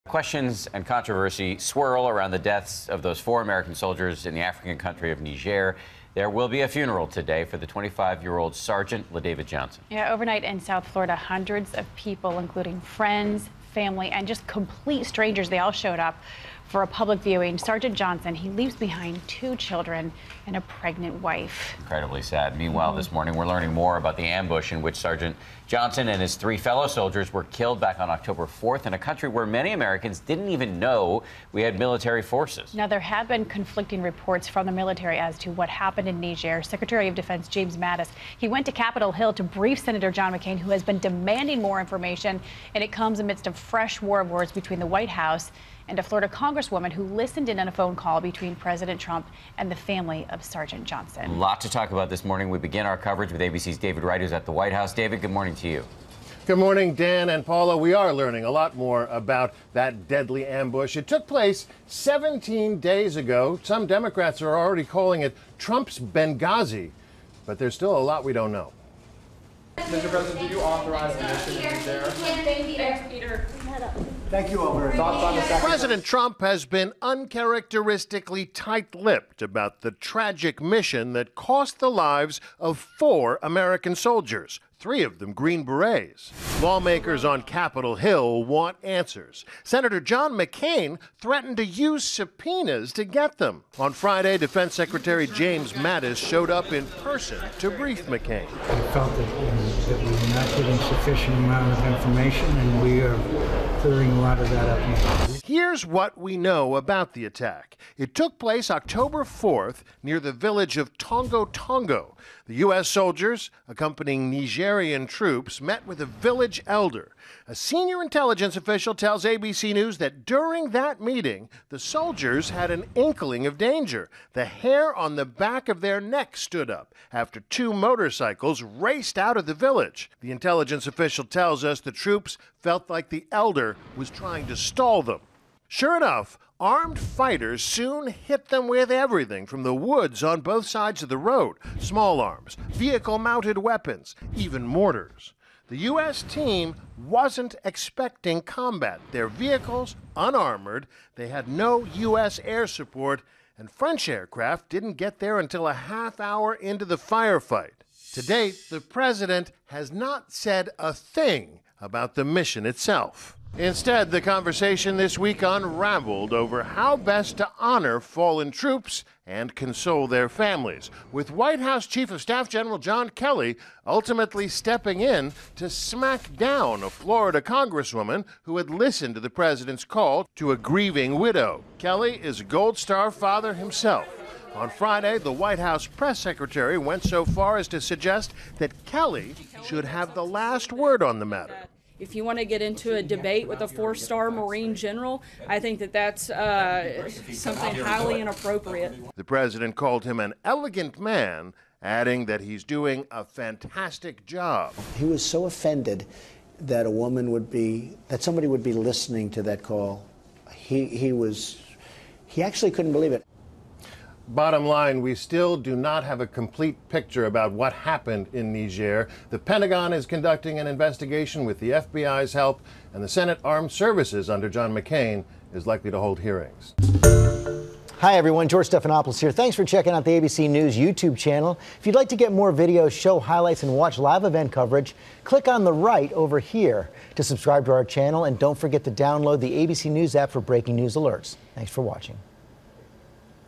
Defense Secretary Mattis briefs members of Congress on deadly Niger mission
The secretary met with personally with Sen. John McCain discuss the mission in Niger that left four American service members dead.